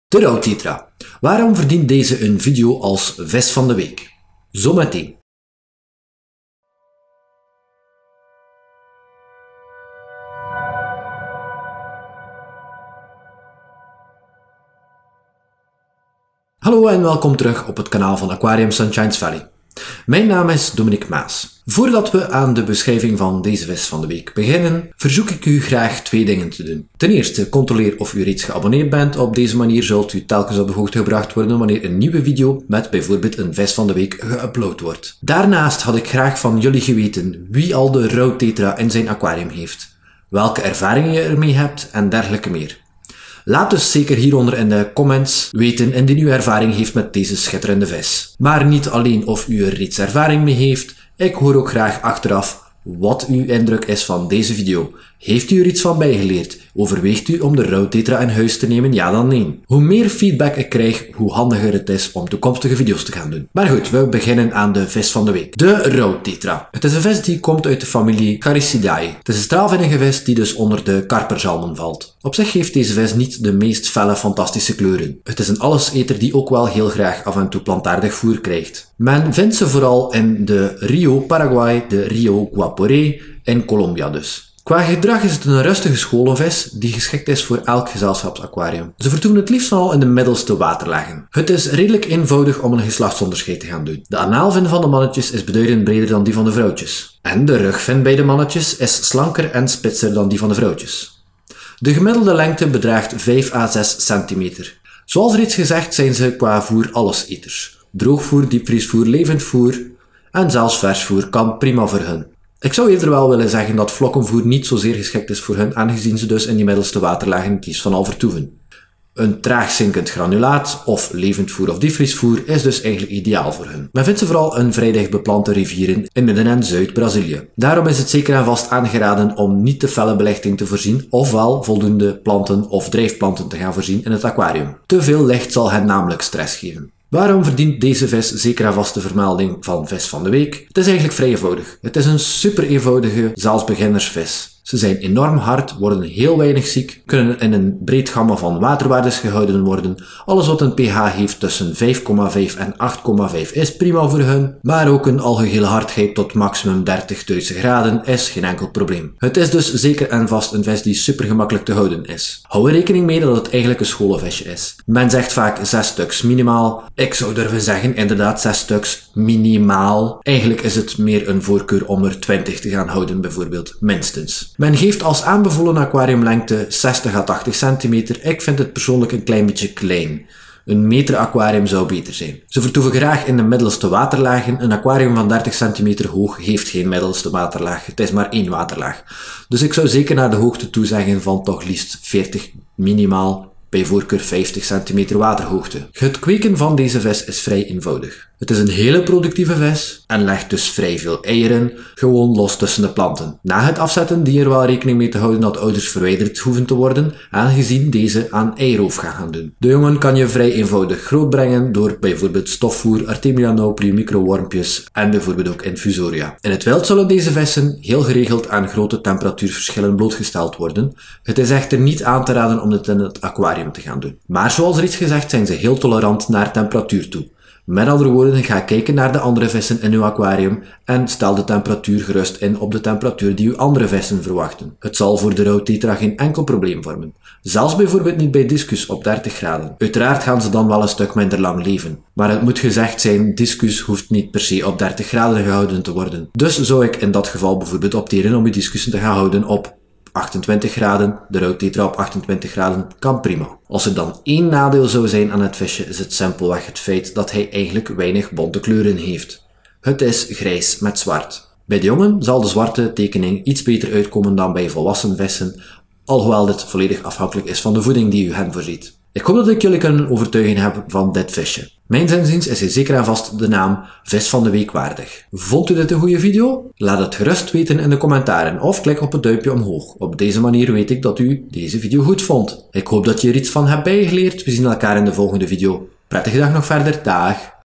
Tropische Aquariumvissen: Rouwtetra - Gymnocorymbus Ternetzi | Aquarium Sunshine Valley
Bezoek mijn nieuwe website/webshop op Rouwtetra - Zwarte Tetra - Gymnocorymbus Ternetzi (black widow tetra)

Het is een bijzonder makkelijk te houden visje die wel tegen een stootje kan. Zijn minder felle kleuren doen dit visje niet voldoende recht aan. Door deze minder bonte kleuren is hij minder populair dan wat hij zou moeten zijn. Het is namelijk een visje die bijzonder sterk is, die bijzonder makkelijk te houden is, die heel veel weerstand heeft naar ziektes toe en die fouten van de kant van de aquariaan wel vrij snel vergeet. Hij kan in principe gehouden worden op aquarium temperaturen tussen de 18 en 28 graden, PH tolerantie is tussen PH 5.5 en 8.5 (dus vrij ruim) alsook de hardheid van het water is hij weinig kieskeurig op: alles wat minder is dan 30DH is prima voor deze vis.

Een bijzonder hardnekkig visje dus, zeker en vast geschikt voor een beginnend aquarium liefhebber, maar ook ervaren rotten kunnen dit visje heus wel op prijs stellen! Aquarium Sunshine Valley